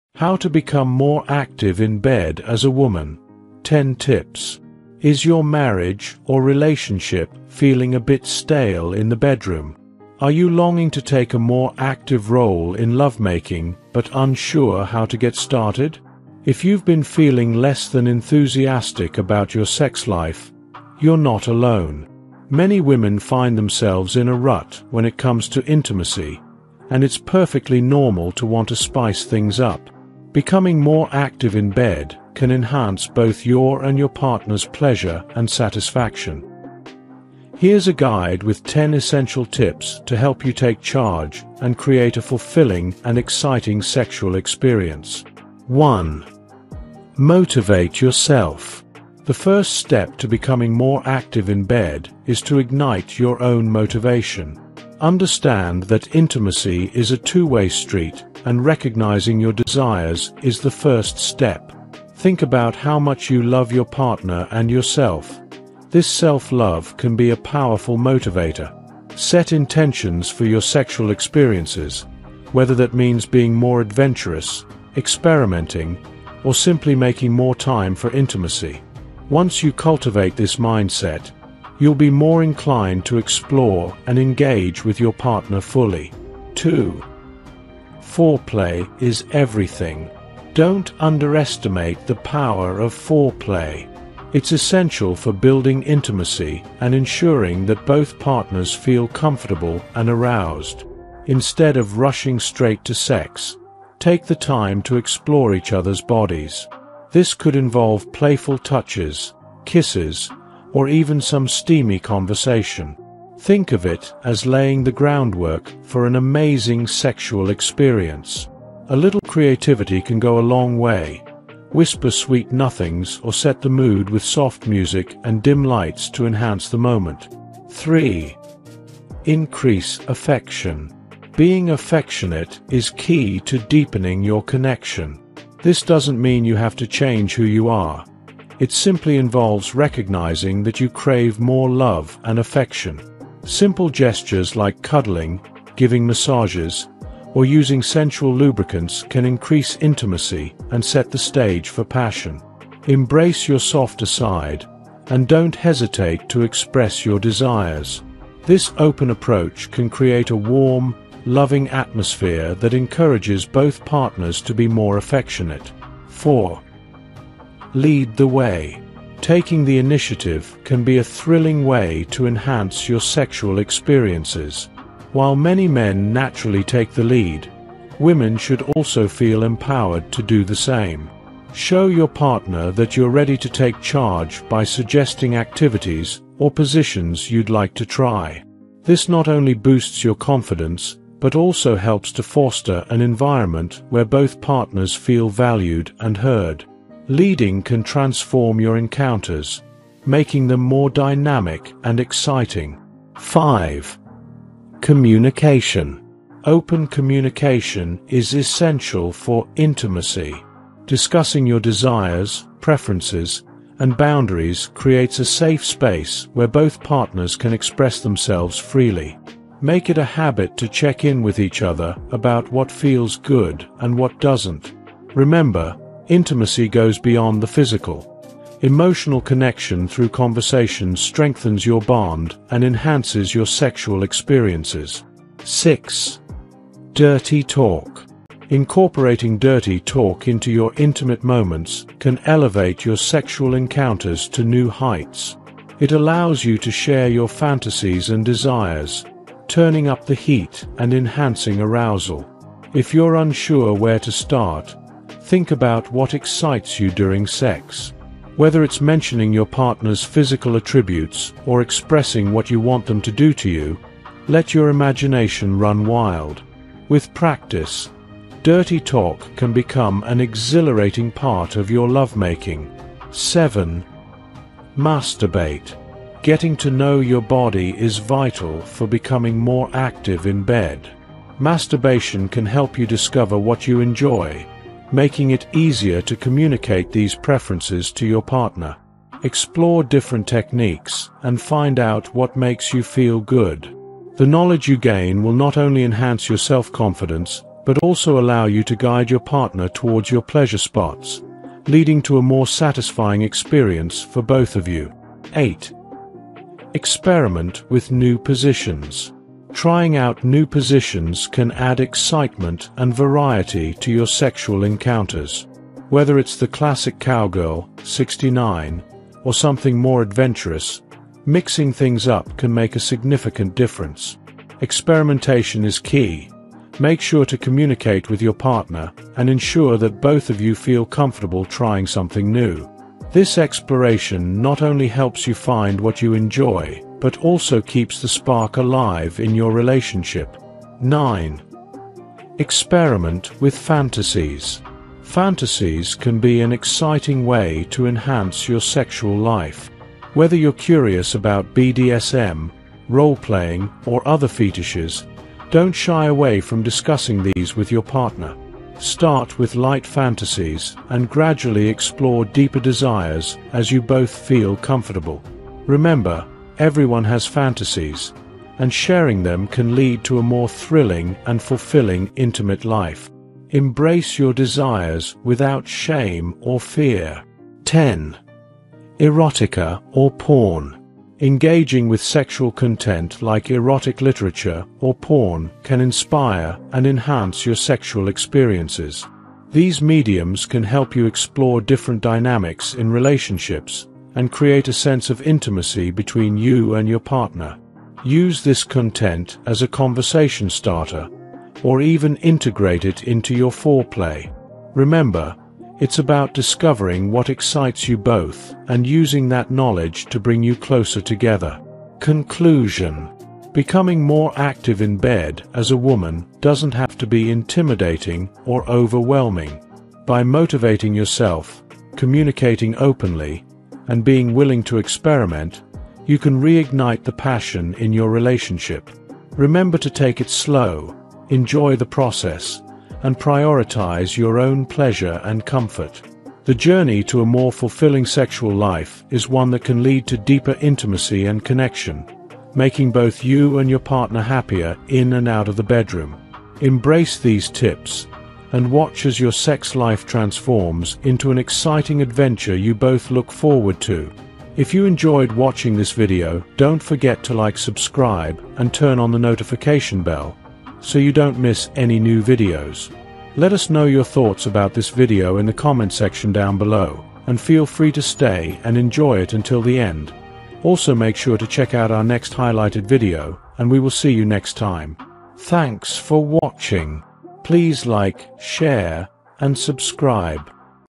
10 Proven Tips for Women to Be More Active in Bed and Boost Intimacy with Your Partner
10 Proven Tips for Women to Be More Active in Bed and Boost Intimacy with Your Partner | Awesome Facts
Are you looking to spice up your sex life and become more active in bed? Whether you’re feeling like things have become monotonous or you want to explore new ways to connect with your partner, this video is here to help. As a woman, it's normal to feel like you want to play a more engaged role in the bedroom. These 10 proven tips will help you become more active, confident, and in control, transforming your sex life and deepening the intimacy with your partner.

1. Motivate Yourself
The first step in becoming more active in bed is to start with motivation. It’s important to connect with your desires and understand what excites you about your partner and the act of lovemaking. Being mentally prepared can fuel your passion and push you to take charge.

2. Embrace Foreplay
Foreplay is the secret ingredient to building stronger intimacy. Take your time to enjoy every moment, from sensual touches to flirtatious words. By focusing on foreplay, you can ignite desire and increase your connection before diving into the main event.

3. Show More Affection
Affection builds a powerful emotional bond. By increasing physical affection such as cuddles, kisses, or even massages, you set the mood for more intense moments. This kind of warmth and closeness makes your partner feel loved and appreciated.

4. Lead the Way
Don’t hesitate to take the lead! While it’s common for men to initiate intimacy, you can surprise your partner by being more assertive in bed. This shows confidence and adds a new dynamic to your relationship, giving both of you the chance to explore each other more fully.

5. Communication Is Key
Open communication is essential for building intimacy. Talk to your partner about your desires, needs, and what makes you feel good. Understanding each other’s preferences will help create a better experience and bring you closer.

6. Get Comfortable with Dirty Talk
Dirty talk can be an exciting way to engage your partner and express what you enjoy. If you’re not used to it, start slow. Try saying things that feel natural and sexy to you, and soon, you’ll feel more confident expressing your desires verbally.

7. Masturbate to Discover Your Preferences
Self-exploration helps you discover what turns you on. By understanding your own body and preferences, you can share this knowledge with your partner, making your intimate moments even more pleasurable.

8. Experiment with New Positions
Don’t be afraid to step outside your comfort zone and try new positions. Whether it’s the cowgirl, 69, or something else, experimenting with different ways of being intimate will keep things fresh and exciting for both of you.

9. Explore Sexual Fantasies
Sexual fantasies are a natural part of intimacy. Talk to your partner about your fantasies and see if they’re open to trying something new. This could involve light role-playing or exploring specific fetishes. Whatever it is, be sure to communicate your boundaries and keep it fun.

10. Incorporate Erotica or Porn
Reading erotic stories or watching porn can be a way to explore new ideas together. It can also add an element of excitement and help both partners get into the mood. Use these materials to inspire your time in the bedroom and discover new ways to connect.

Being more active in bed is about building confidence, deepening intimacy, and exploring new experiences with your partner. By applying these 10 tips, you’ll feel more in control of your sex life and bring a renewed energy into your relationship. Whether you’re adding more affection, trying out new positions, or leading the way with sexy communication, these steps will ensure you enjoy every moment and make it memorable for both you and your partner.